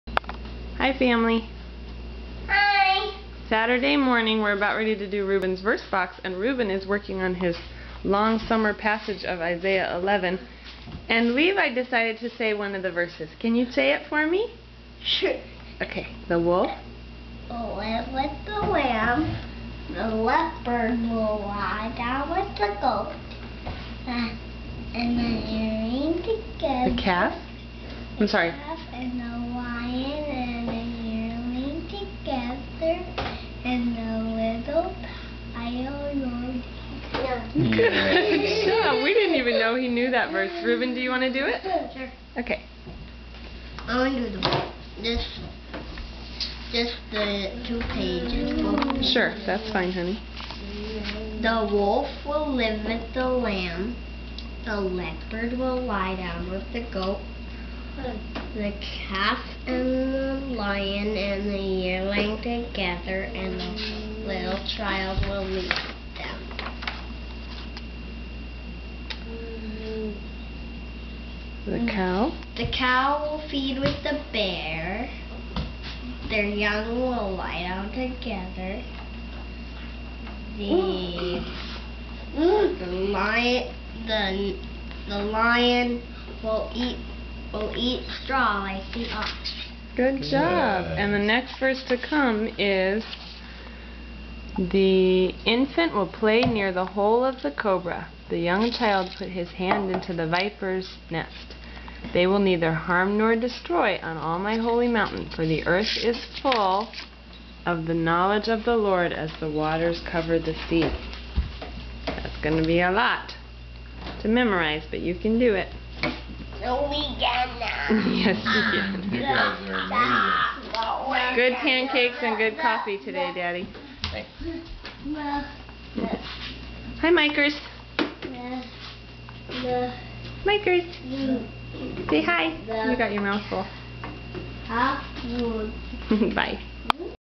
Isaiah 11
The boys saying verses on Saturday morning.